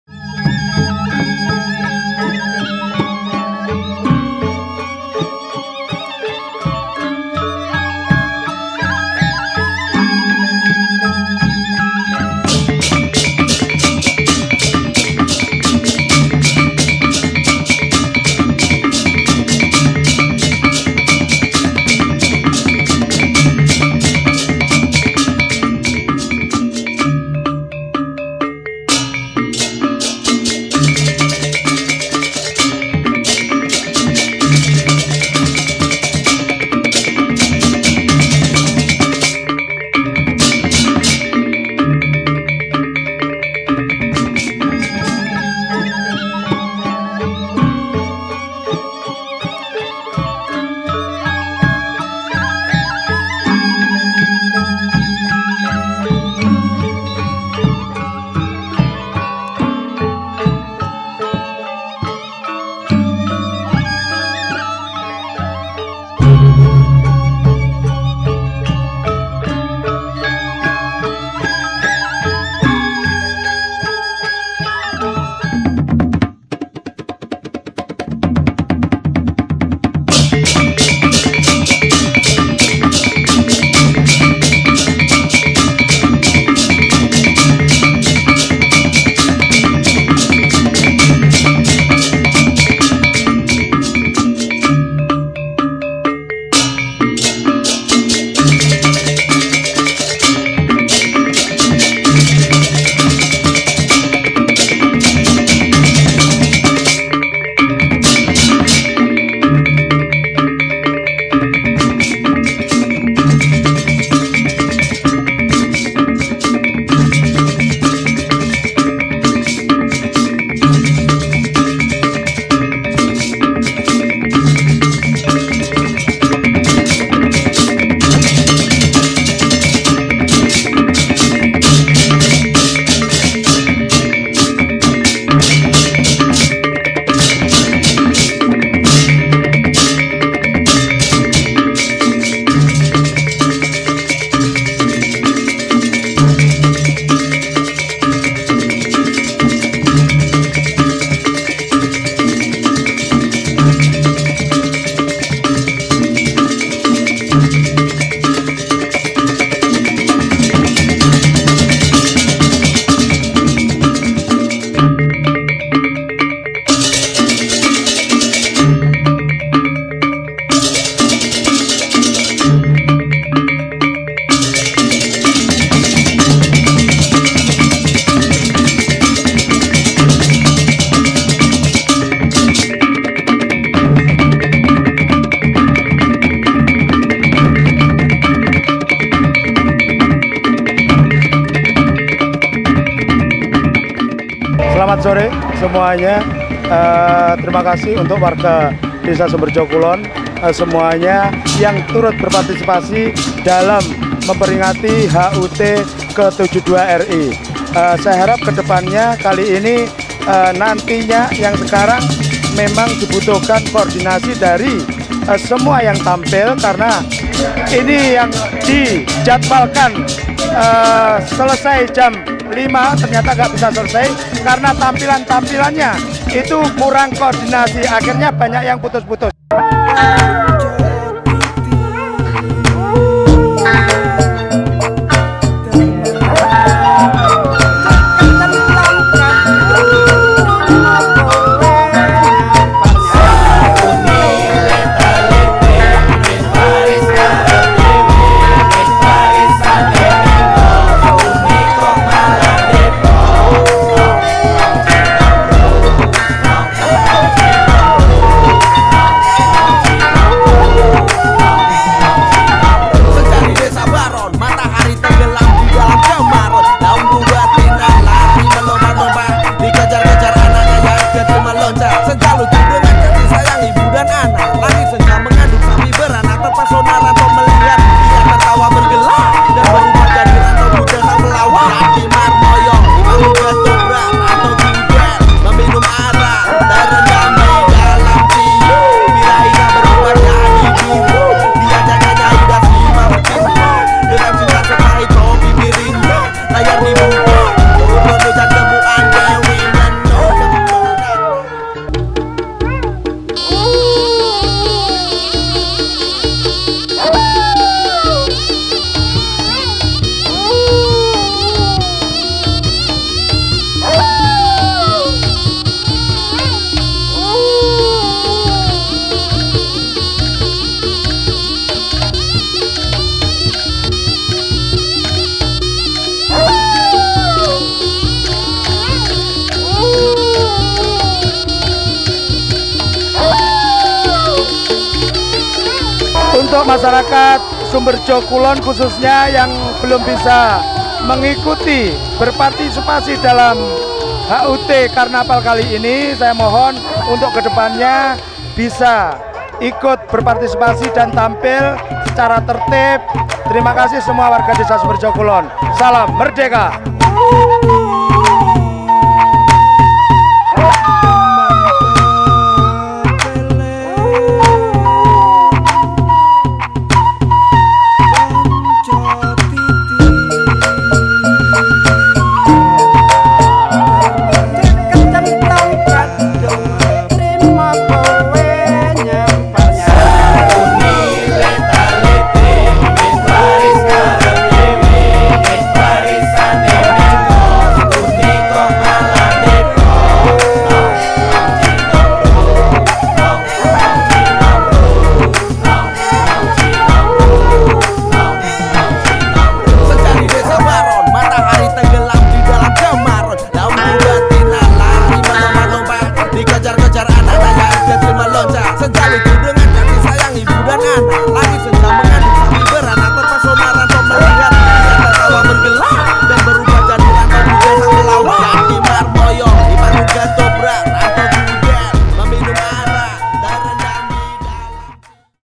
HEBOOH... Karnaval Ds. Sumberjo kulon - Ogoh" Hanoman gendong Dewi Sintha & Nogo Barong beraksi!!
KARNAVAL SUMBERJO KULON 23 SEPTEMBER 2017
Bersama Salon VIRA - Sumberjo Kulon (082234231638)
               featuring
HNS DIGITAL MOVIE - Kaliwungu (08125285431)